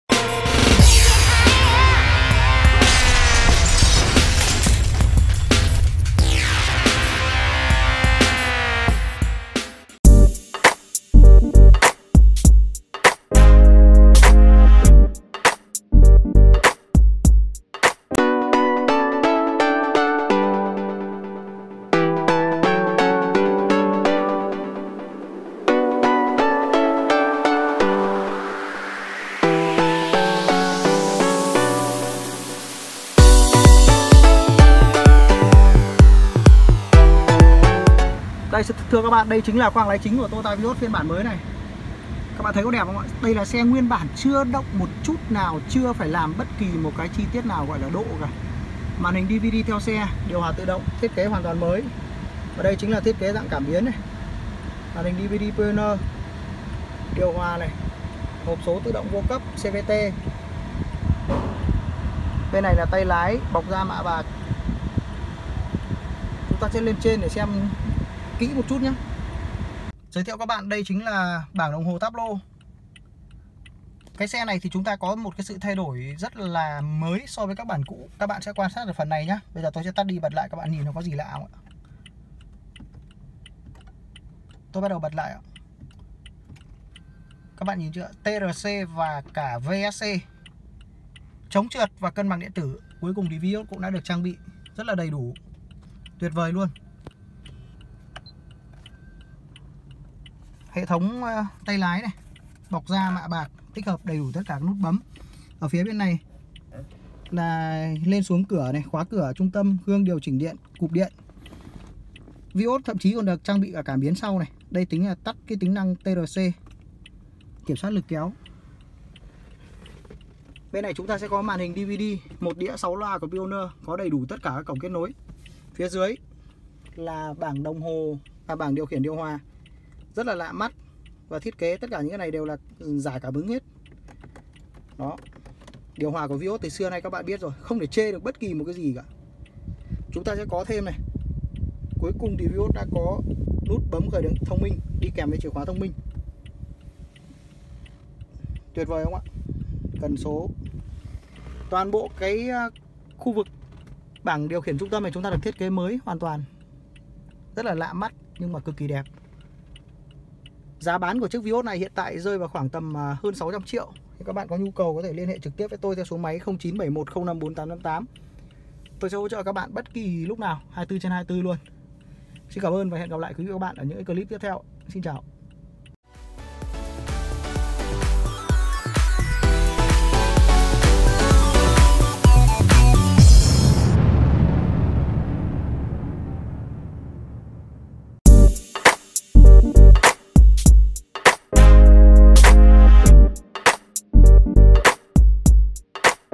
Trải nghiệm nội thất Toyota Vios 2020 hoàn toàn mới vừa ra mắt, nhiều nâng cấp giá trị
Hyundai Lê Văn Lương - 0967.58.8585

Xem chi tiết tại Toyota Vios 2020 có giá từ 490 triệu, hỗ trợ trả góp 80%.